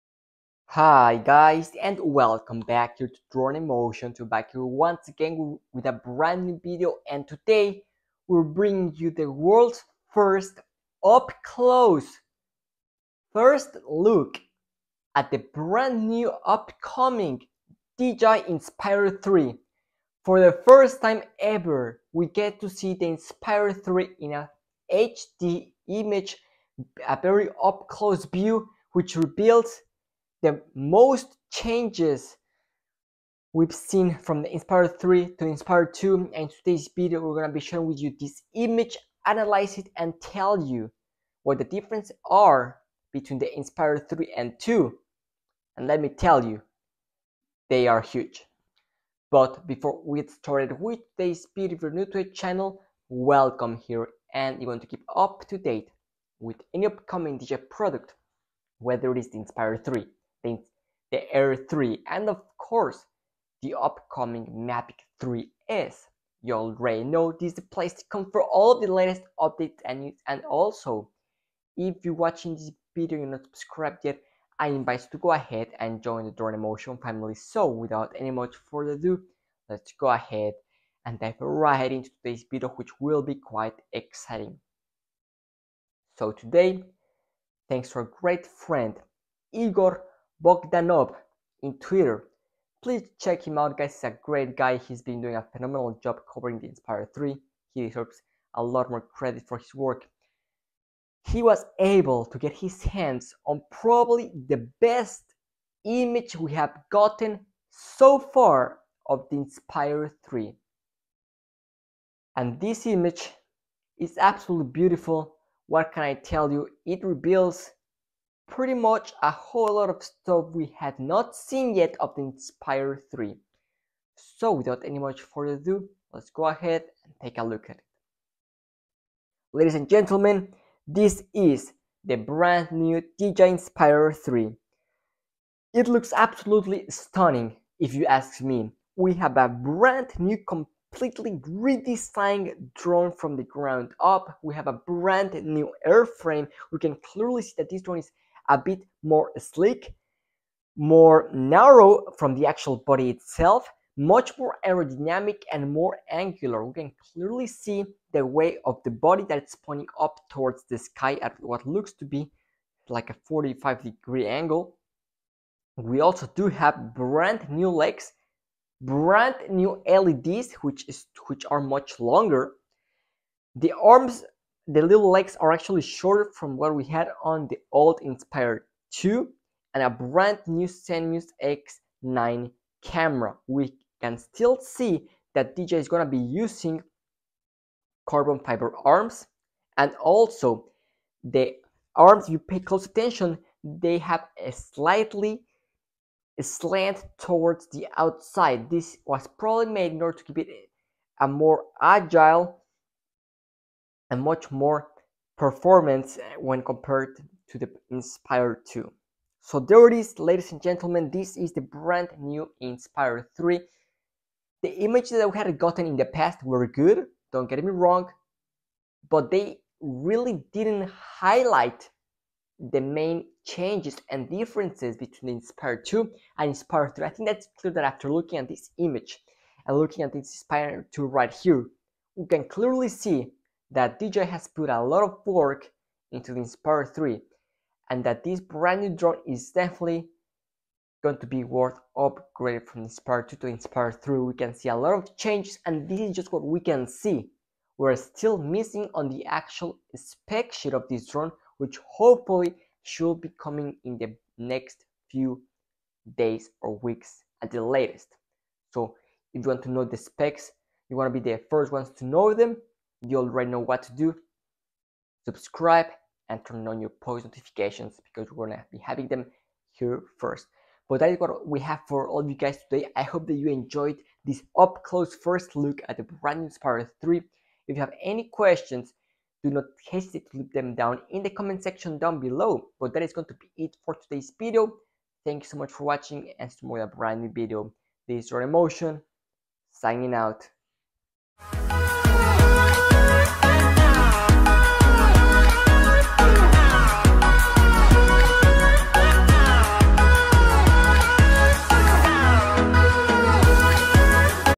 DJI Inspire 3 Up Close Look
In today's video we take a closer at the Inspire 3.